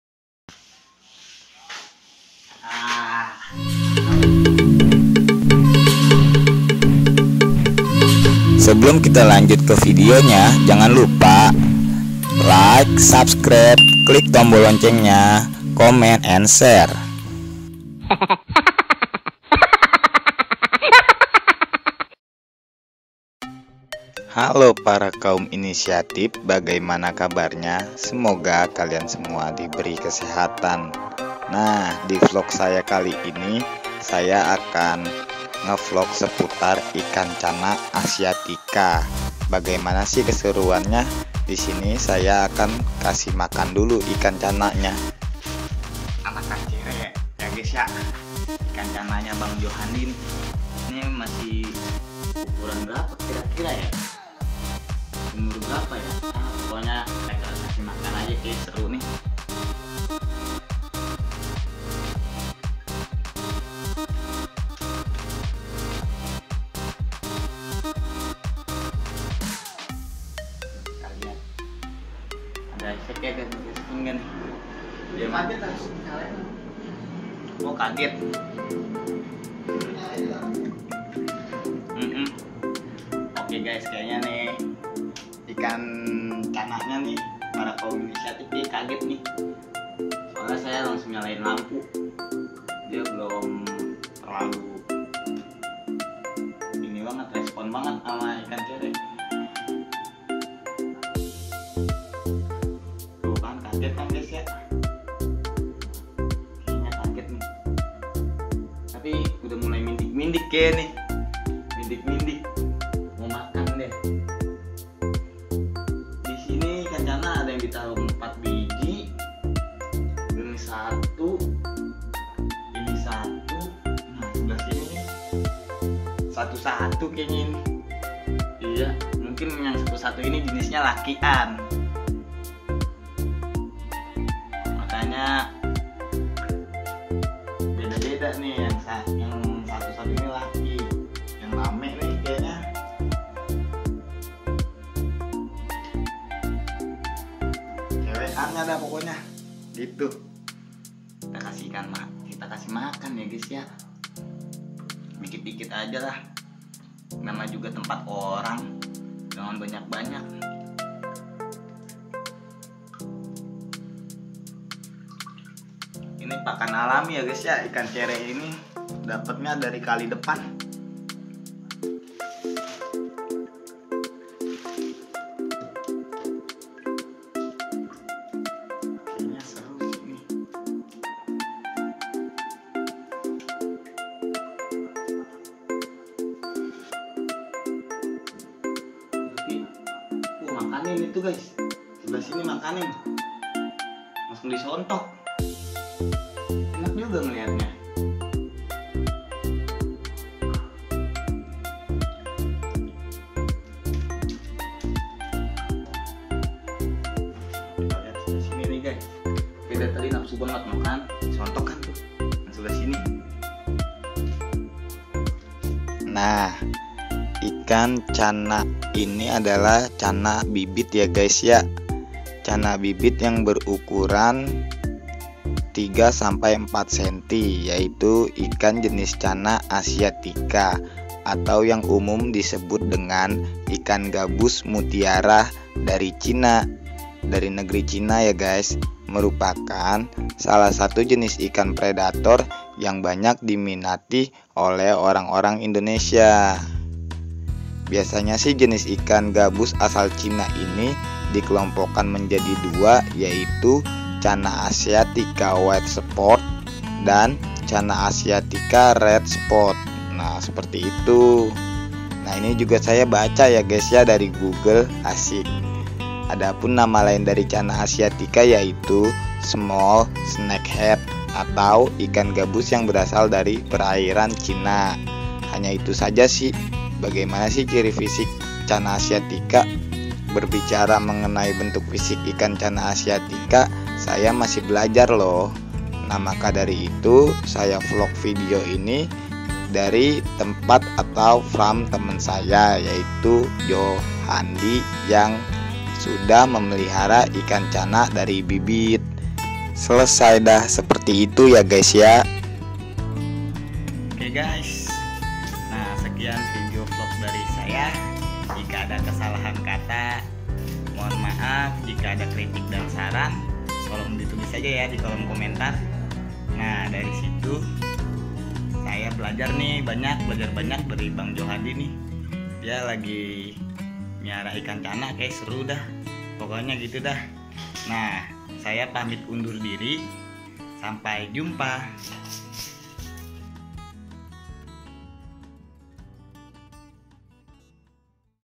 Ngumpanin Bibit Ikan Channa Asiatica Si Predator Air Tawar Pakan alami Ikan Cere ‼️
Vlog ngumpanin Bibit Ikan channa asiatica Si Predator Air Tawar pakai ikan cere, ikan gabus china

Video kali ini berisikan kegiatan Tengokin bibit ikan channa Asiatica, edukasi tentang perikanan air tawar

Pengisi suara saya sendiri

Cameramen saya sendiri

Kamera yang di gunakan handphone honor 9 lite

Mikrofon langsung dari handphonenya

Musik di dapat dari inshotapp.page.link

Aplikasi editingnya saya menggunakan inshotapp.page.link

Semoga bermanfaat